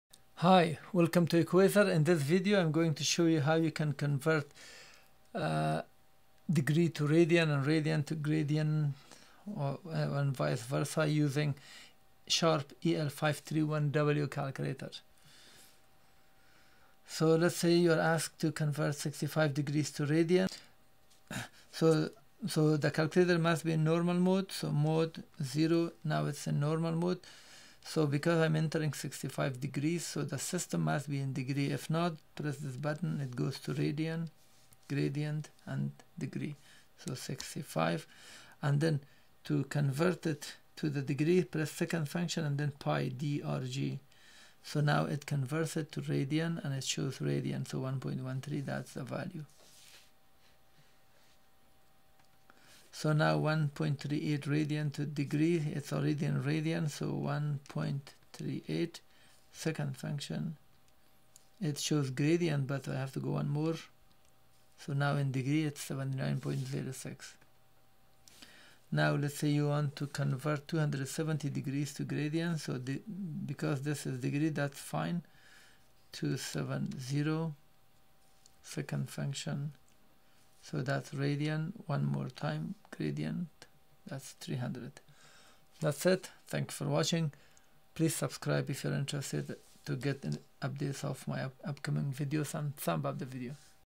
Sharp EL-531W convert angle degree to radian
In this video show you how to

-Convert degree and radian to one another.

Purchase Sharp EL-531W from Amazon

Using Sharp EL-531W Scientific Calculator.

Tutorial by Equaser.